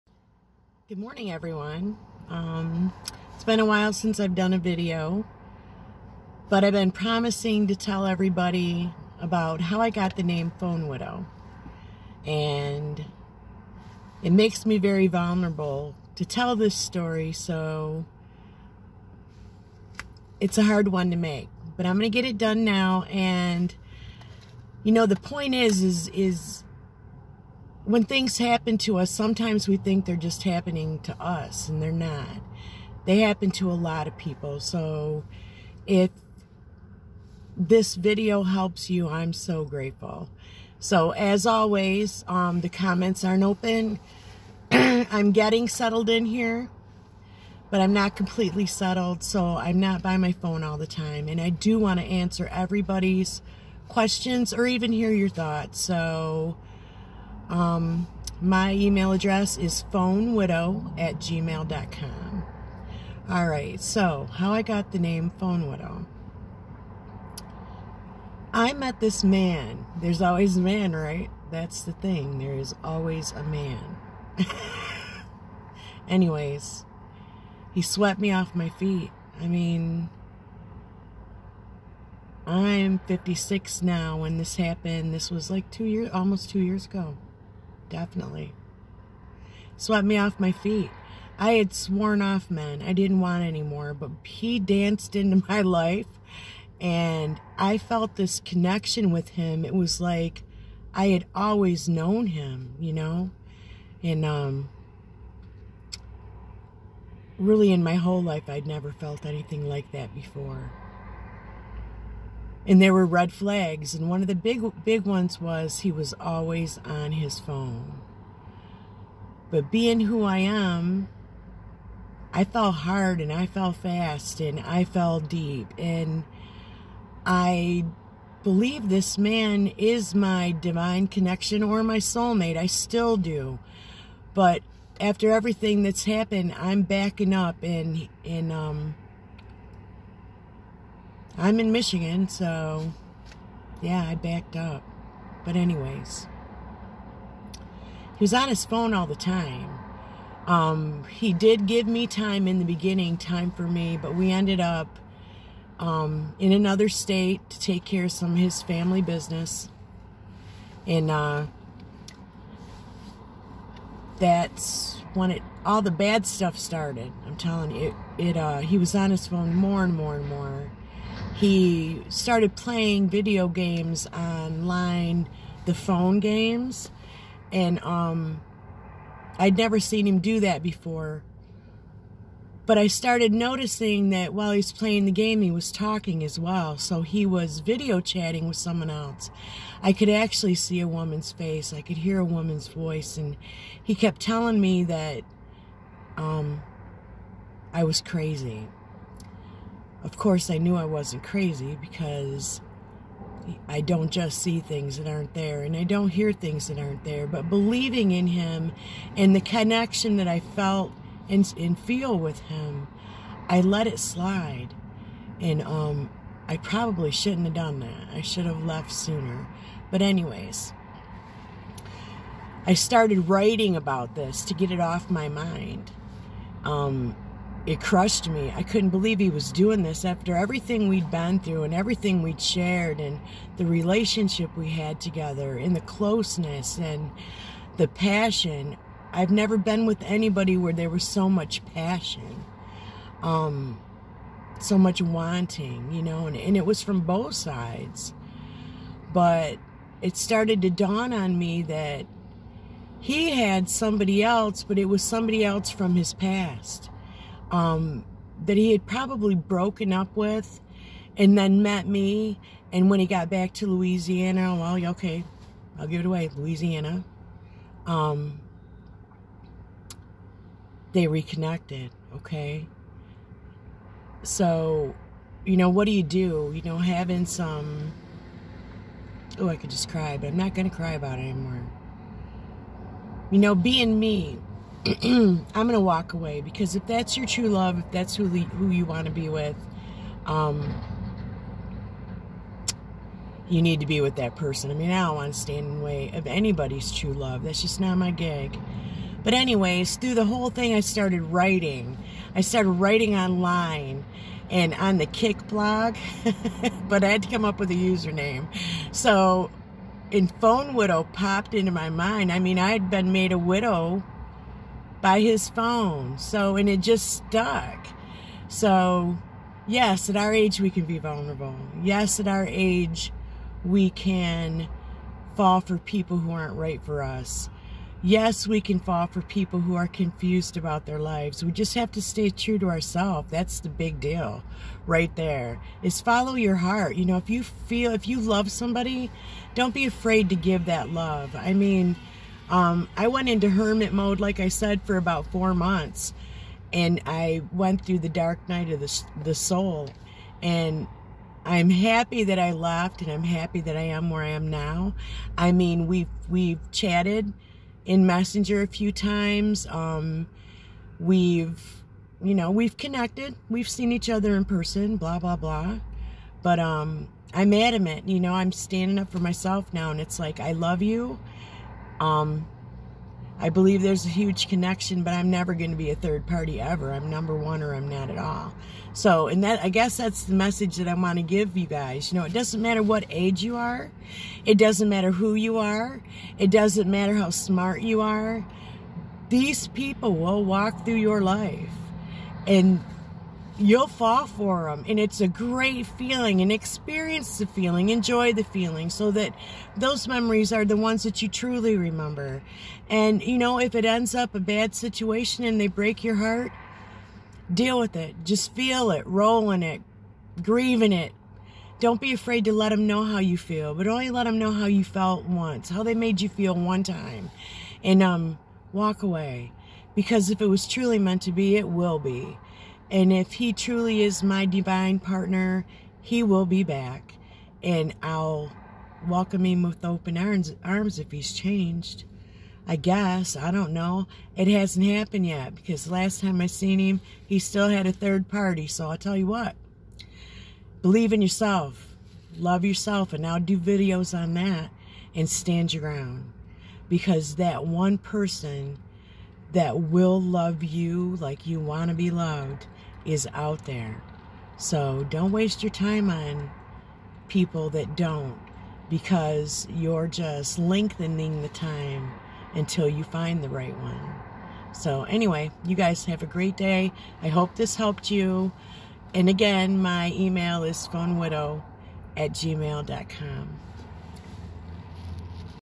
How I became Phone Widow
!!!Comments are now opened!!!
I'm sparin you all the grimy details. This video is not to say anything bad about anyone..I give the basic details of my experience.  The purpose is to help others grow and learn from my experiences